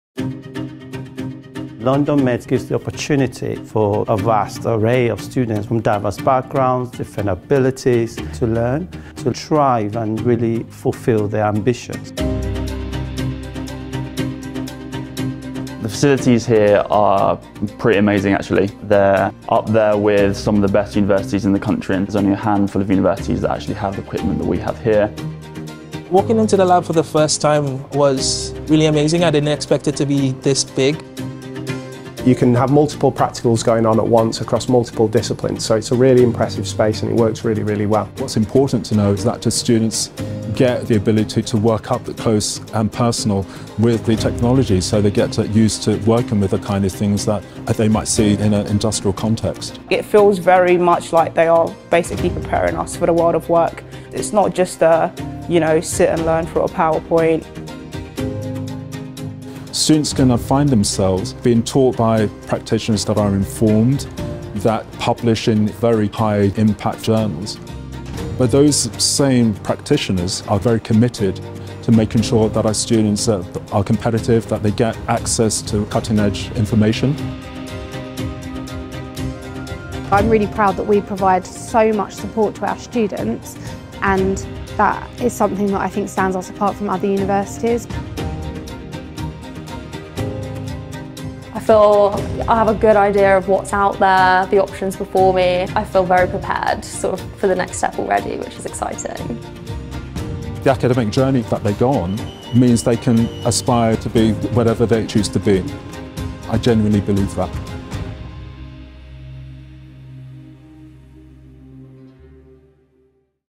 Study a science degree at London Met
By choosing to study a science degree at London Met, you'll have access to our £30 million Science Centre – home to a superlab, which has 280 individual workstations and is one of the largest and most advanced science teaching facilities in Europe, as well as state-of-the-art sports science and therapy facilities, dietetics labs and more!

Apply today and join our outstanding science community!